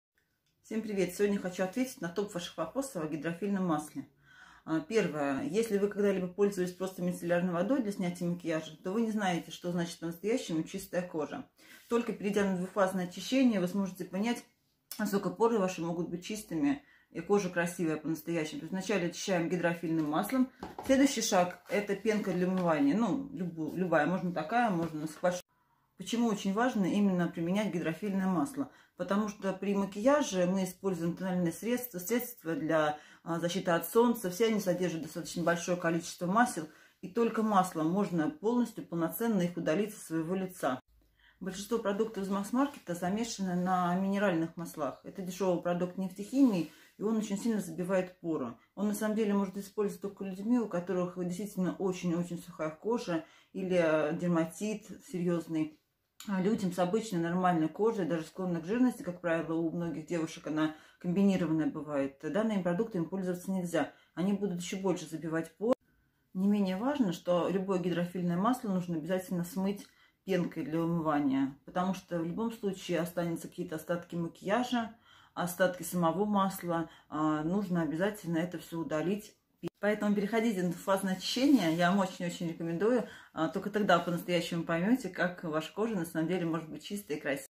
8 февраля 2021 г.
Все о гидрофильном масле
Использование гидрофильного масла поможет очистить поры, ваше лицо приобретет ухоженный и свежий вид